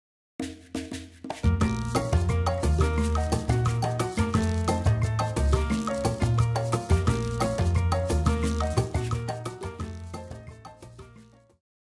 Aerosoles - What To Pack For The Beach
Heading to the beach? Here are our beach essentials for a weekend getaway to the beach!